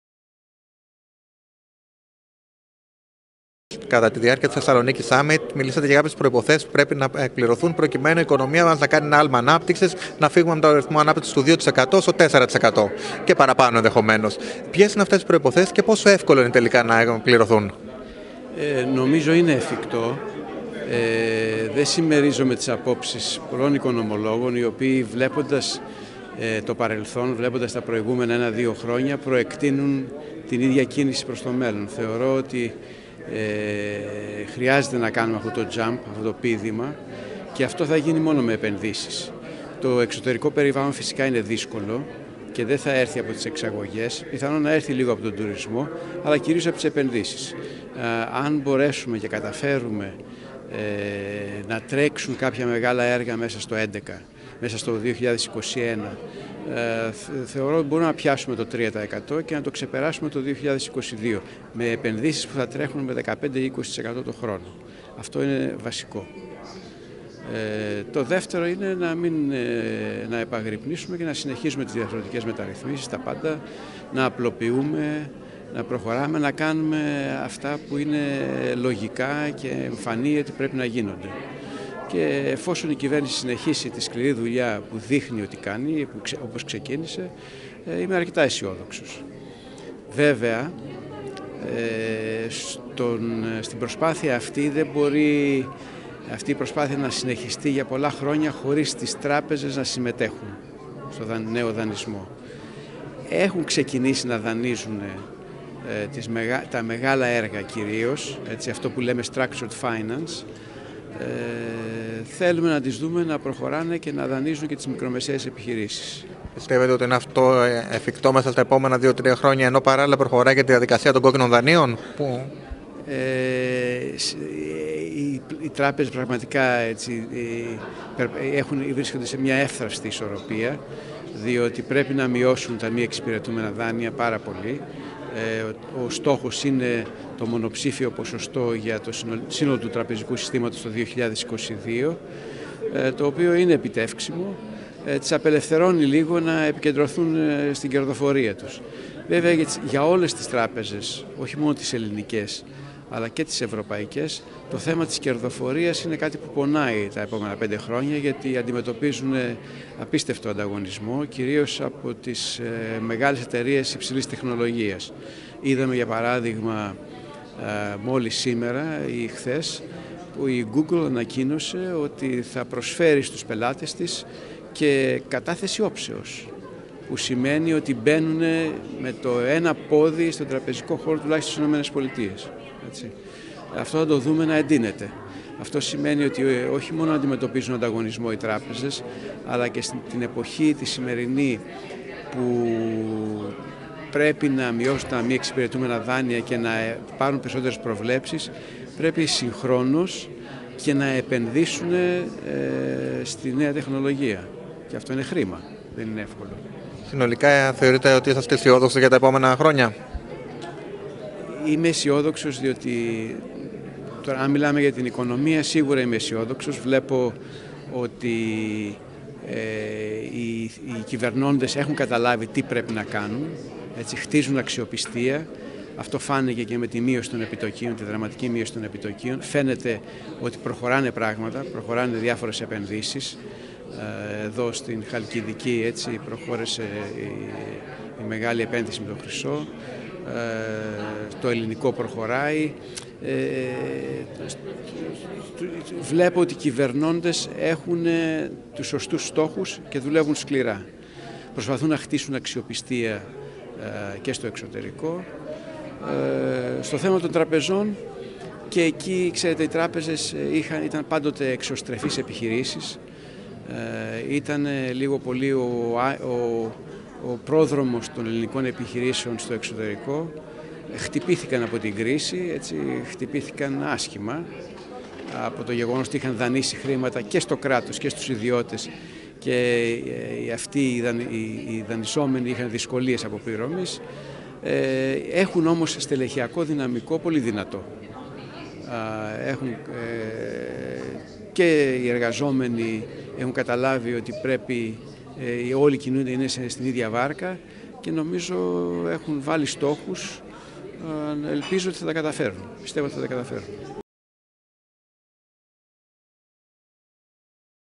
Γκίκας Χαρδούβελης: Μόνο με επενδύσεις θα γίνει το άλμα ανάπτυξης
Ο πρώην υπουργός Οικονομικών μιλά στο insider.gr μετά την ομιλία του στο Thessaloniki Summit